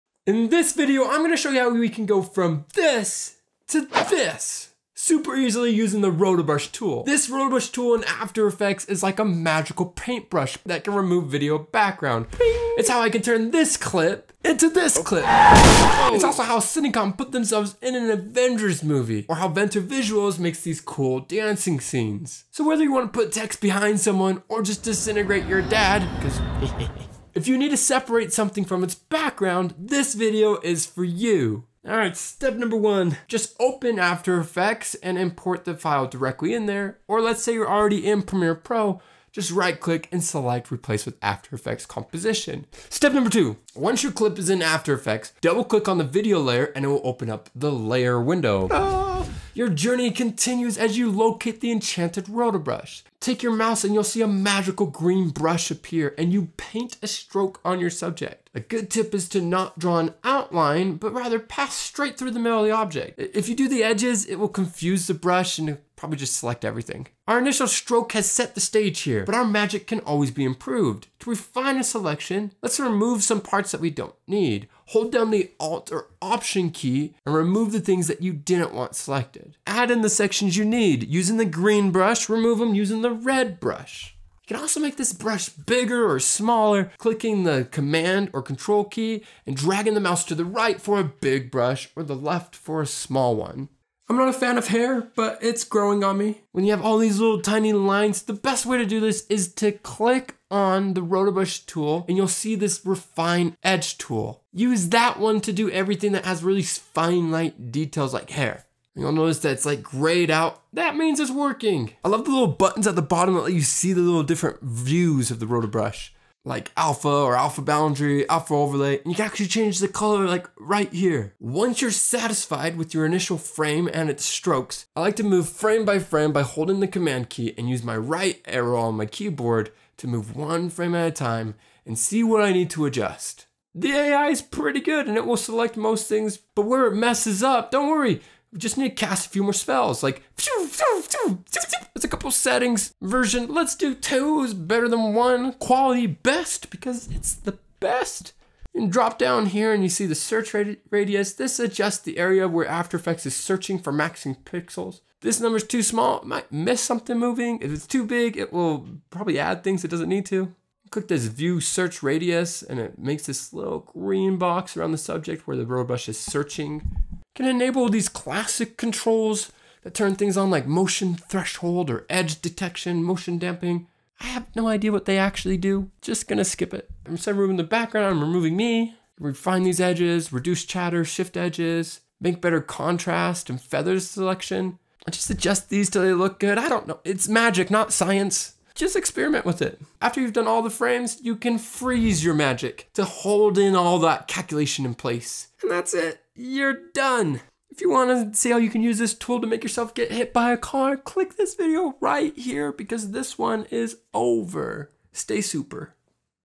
How to Remove video background EASY After Effects
Rotoscoping with the rotobrush tool has made it TOO EASY!

In this video, you'll learn how to use the rotobrush tool found in After Effects

If you hate the video, I still won't turn away your money. :D

Step 1: Opening after effects

You have two options here
, you search for after effects in your creative cloud, and import your file directly in there
You’re already working in Premiere Pro and you right click, select replace with after effects composition.
PRO TIP: Many people shoot at  60 frames per seconds, which is like taking 60 pictures every second to make a video. This is great when you need slow motion.  However, most videos are exported and edited at 24 frames per second or 30 frames per second.
So if you have a video at 60 frames per second, but your timeline settings are at 24 frames, nest the clip first before moving it to after effects. By nesting the clip, you’ll only need to edit 24 frames instead of 60!

Step 2: Find the Roto Brush
Once your clip is in After Effects, double click on the video layer and it will open a layer window. Look at that.
Our journey continues with locating the enchanted Roto Brush. It's hidden here in the toolbar, Ta-da! The Roto Brush is activated before our eyes.

Step 3: Cast the spell
Time to cast our first spell! Take your magical green brush and paint a stroke on the subject, object, or region you want to remove. A good tip is to not draw an outline but rather pass through the middle of the object. The edges tend to confuse our magic.

Step 4: Refining the Spell
Our initial stroke has set the stage, but our magic can always be improved. To refine our selection, we can add more strokes or even remove some parts. Hold down the Alt (or Option) key to remove portions of the selection with background strokes. Green is selecting, Red is Removing Selections. You can make the brush bigger or smaller, by clicking Crtl (or CMD) and dragging the mouse to the right for a big brush, and the left for a small one.

You can make the edges of things like hair look better using the refine edge tool. It's easy to find - just click and hold the roto Brush icon and a menu will appear. Choose the Refine Edge tool from there.

After you've traced the fine edges you want to improve, you'll notice that the grayed-out area, that means it’s working.

Then freeze your roto and Export.
boundary or alpha overlay, which you can actually change the color of using this button here.

Step 5: Unleashing the Magic
Once we're satisfied with our initial frame and strokes, it's time to unleash the full power of the Roto Brush. I like to move frame by frame by clicking Command (crtl) + Right arrows and going one frame at time, As our magic spreads, it may occasionally include unwanted regions or miss some parts of the original selection. Fear not! To correct these imperfections, we need to cast more spells. Pshew, sphew sphew. Zip Zip.

Step 6: Freezing the Magic
After our magical propagation has worked its way through all the frames, we reach a moment of decision. We can freeze the magic using the Freeze button located at the bottom of the Layer panel. Freezing locks our spell in place, allowing us to make adjustments without re-calculating. We can always unfreeze it and make our changes.